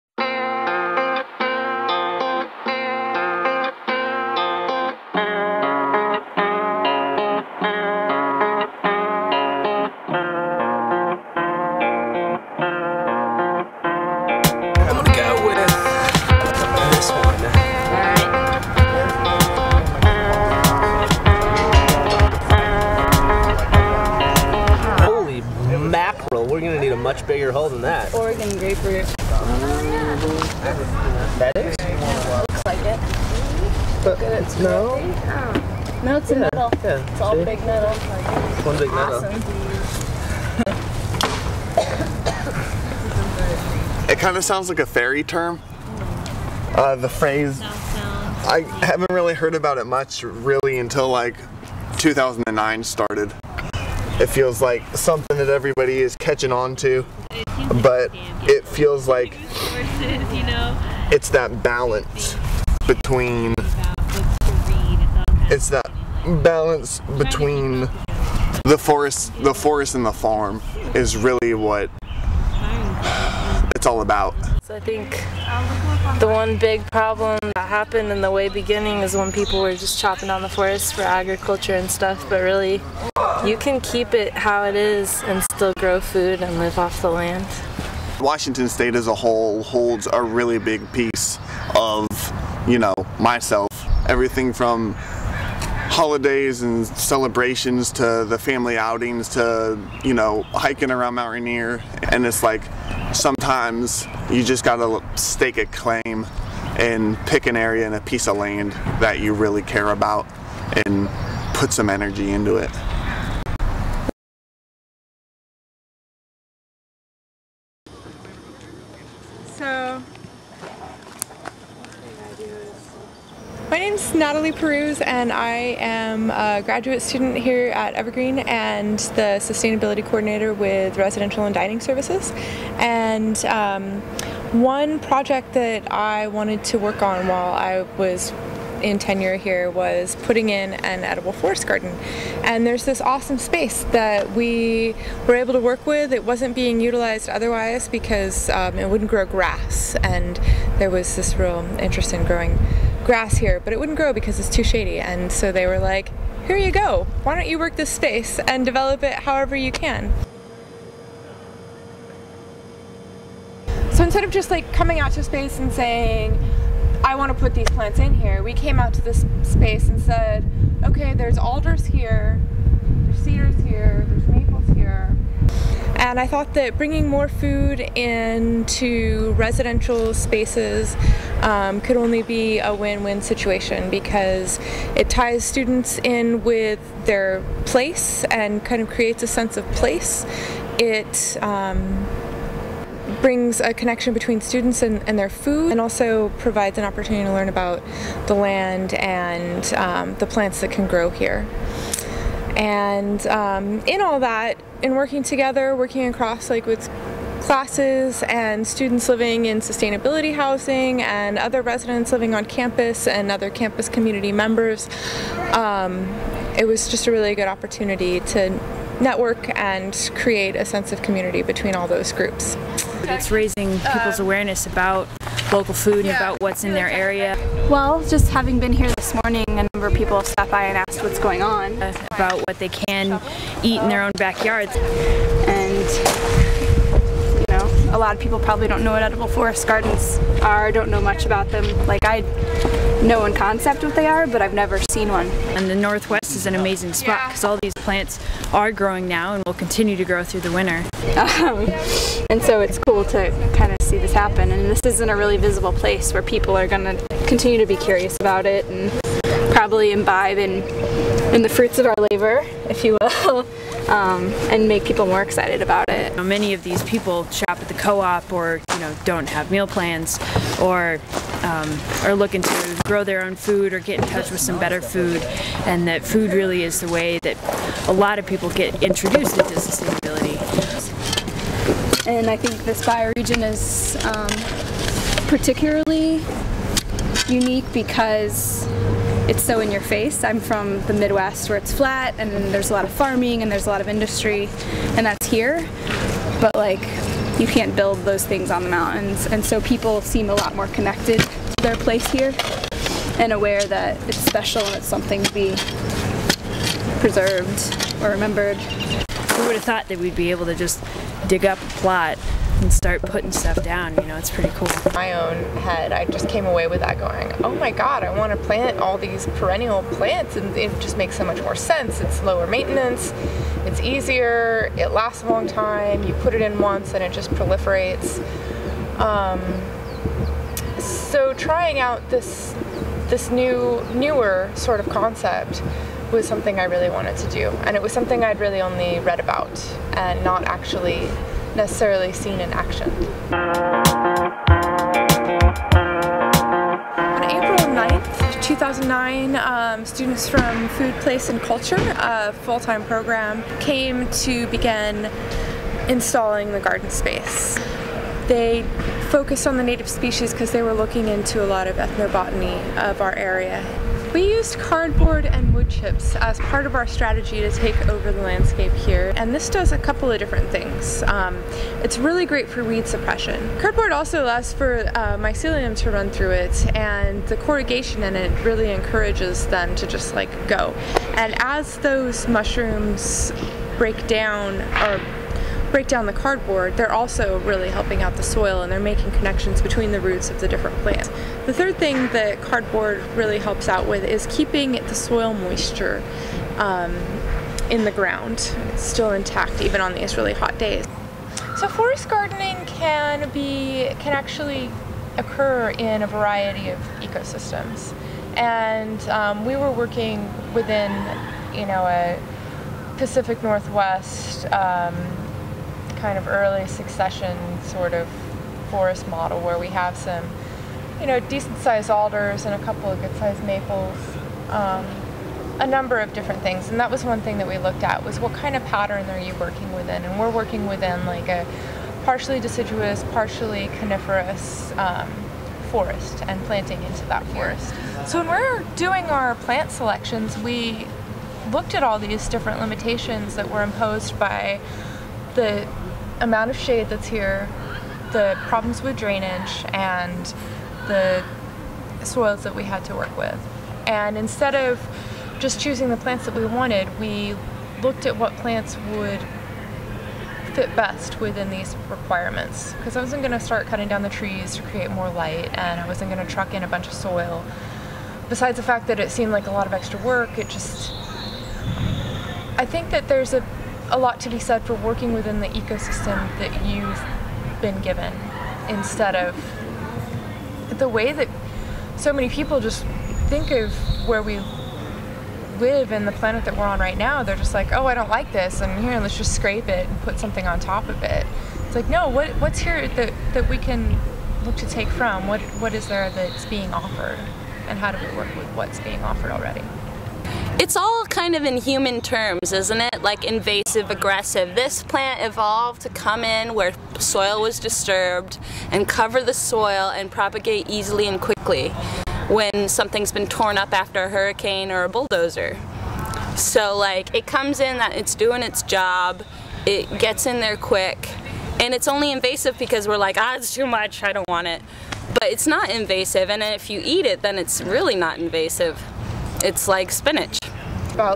Systems of Change: Edible Forest Gardening, part 1
Documentary of edible forest garden installation at the Evergreen State College by filmmaker Phred Swain-Sugarman. Edible forest gardens are perennial polycultures of food-bearing and other useful species. Includes interviews with volunteers and project coordinator. (Part 2)